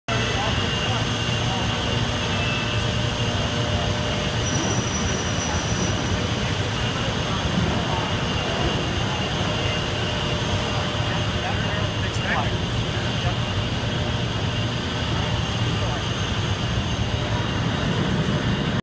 Artprize7 - Steelcase packaging
In this 2nd part of our walking tour, we reviewed the packaging and shipping sections of the floor.